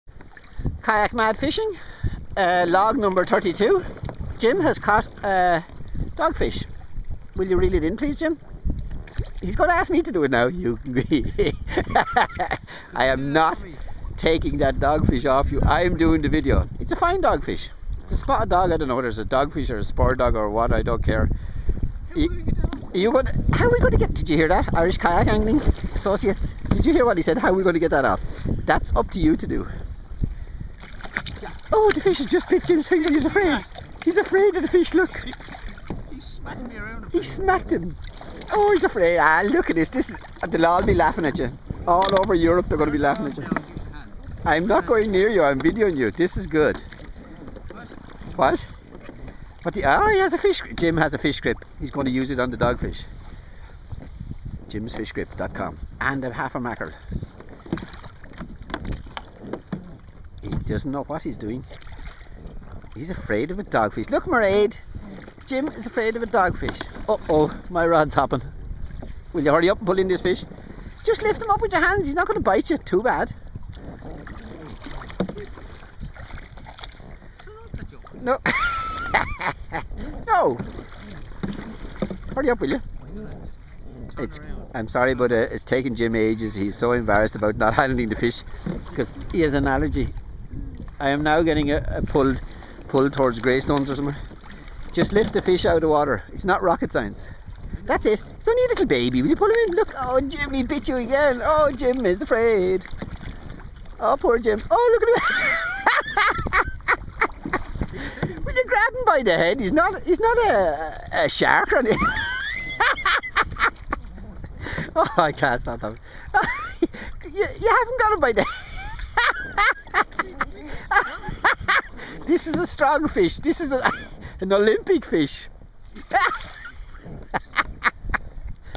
jim catching a dogfish
jim having a fight with a dogfish